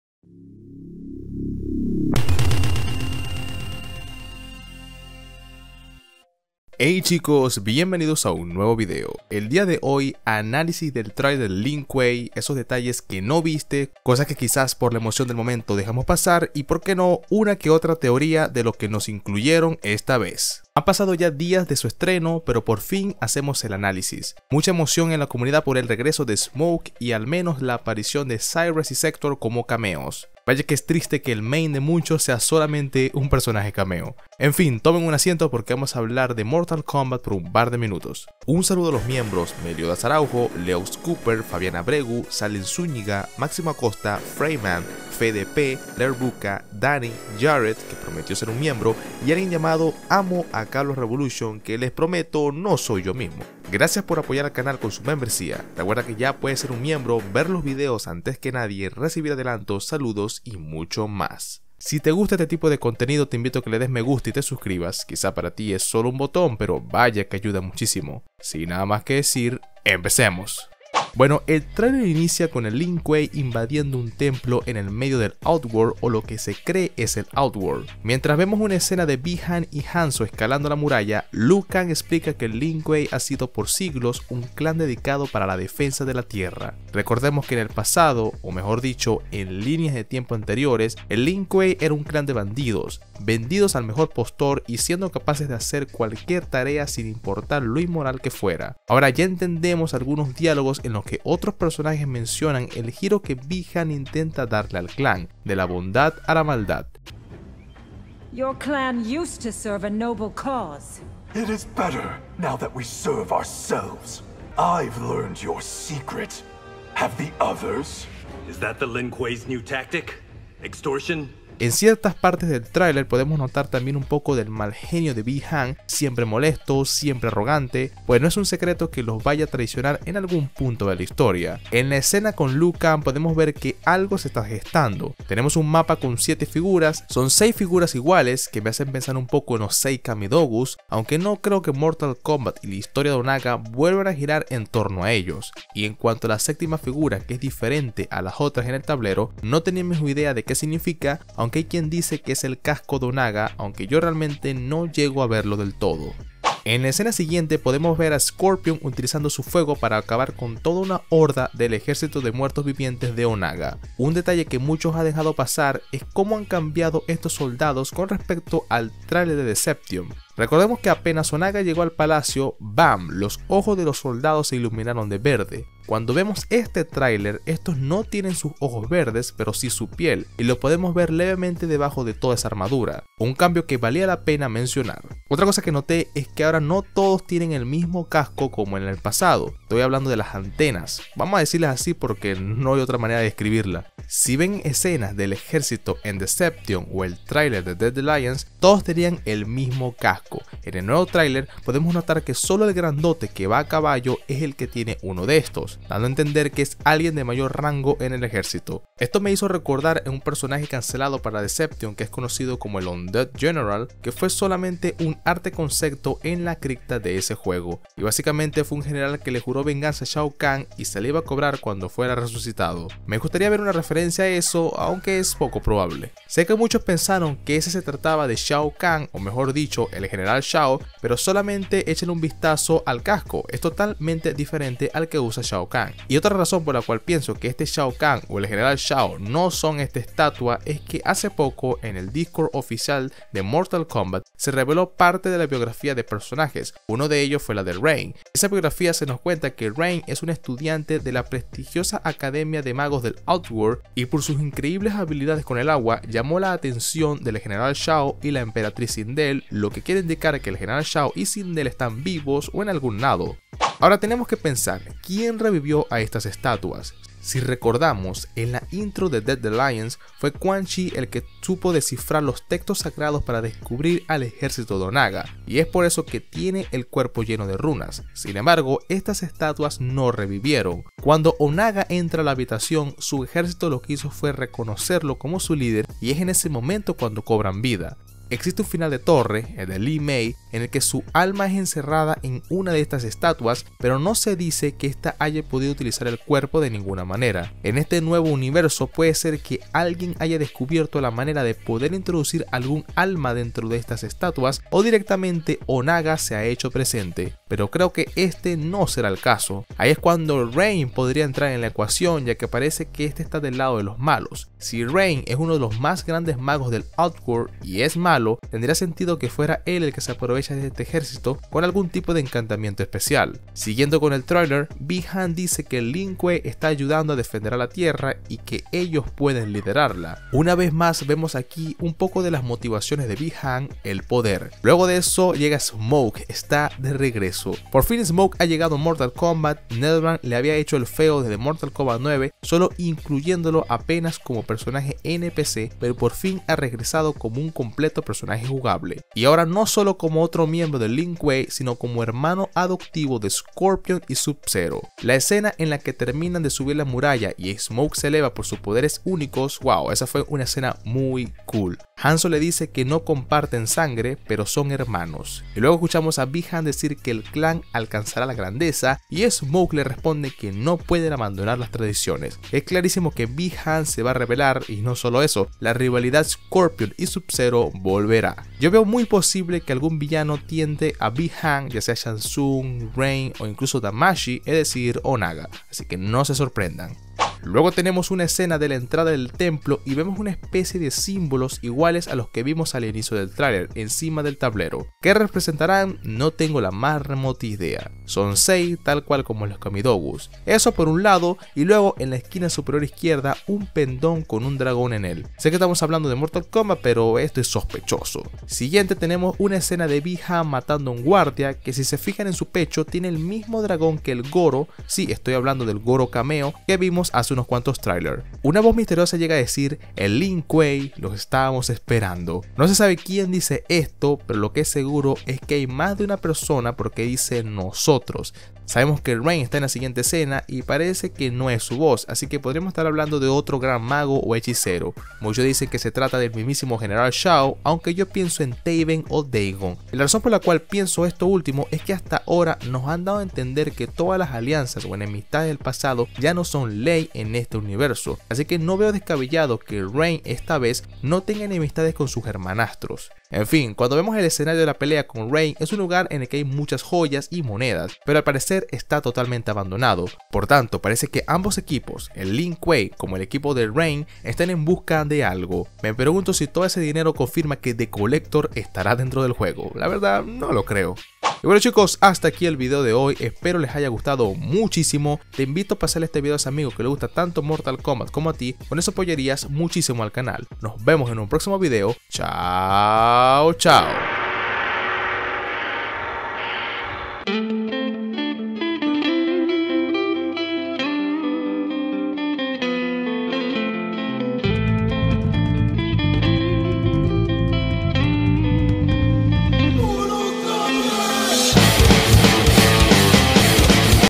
¿HAVIK controla el ejército de ONAGA? La Nueva Alianza Havik y Rain - Shao Kahn es ahora GENERAL
Havik podría estar de regreso, esta vez controlando el ejército de Onaga para su uso personal, y creo que Bi Han se pondrá del lado de ellos. Shao Kahn es revelado como General de la Emperatriz Sindel y parece que Onaga fue asesinado por otra persona.